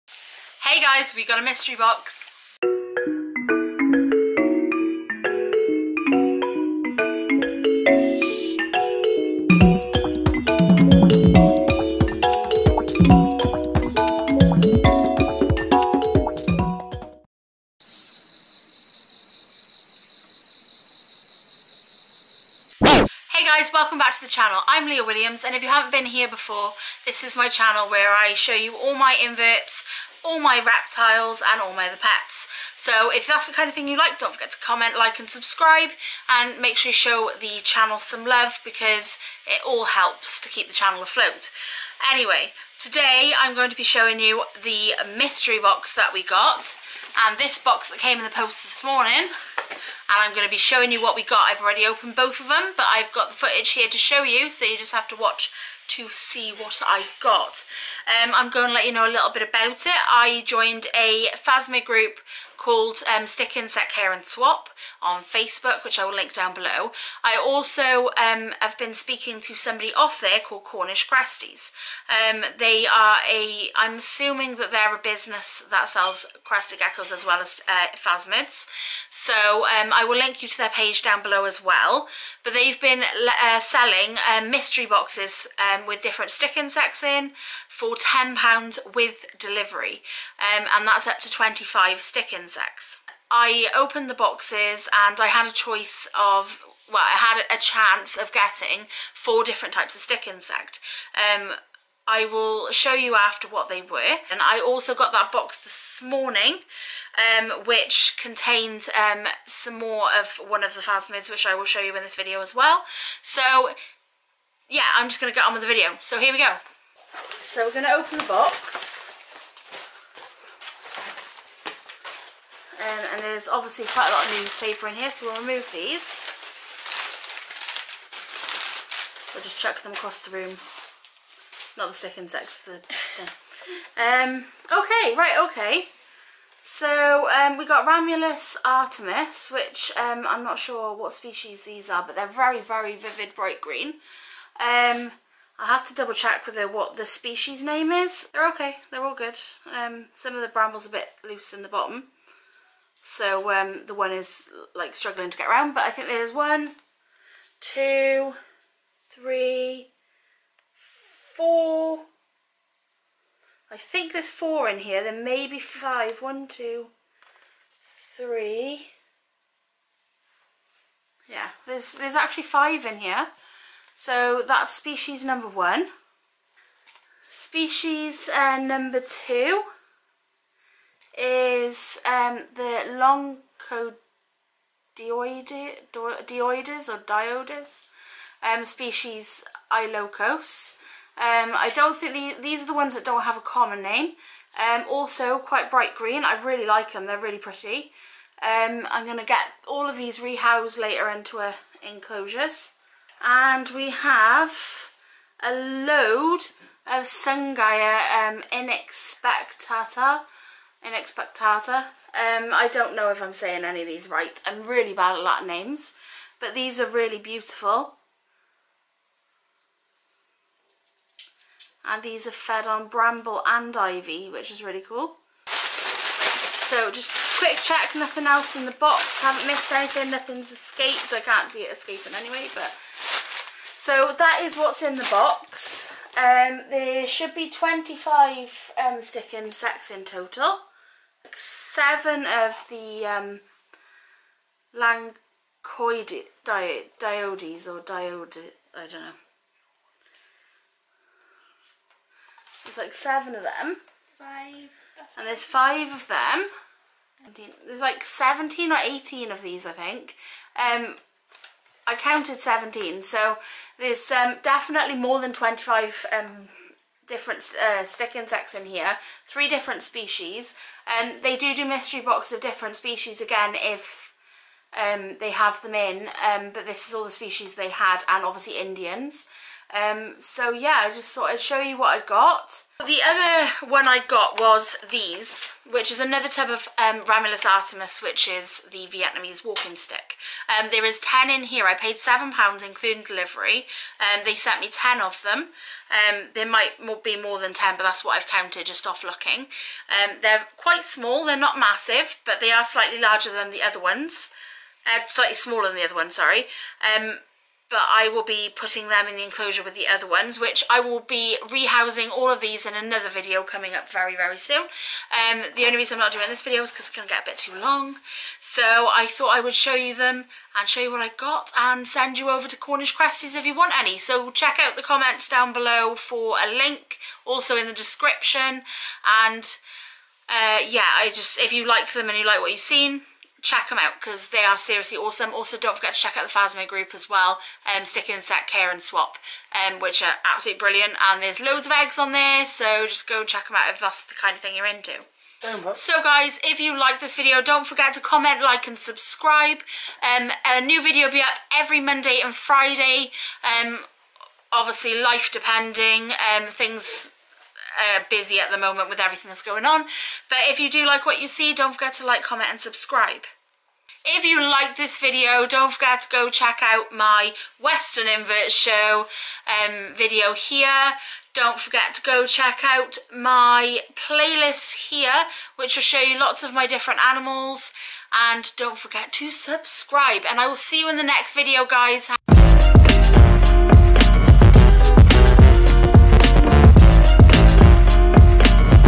Mystery box unboxing from cornish cresties
This mystery box unboxing from cornish cresties I'd the first one I've bought and I cant recommend them highly enough this mystery box was awsome to find out what was inside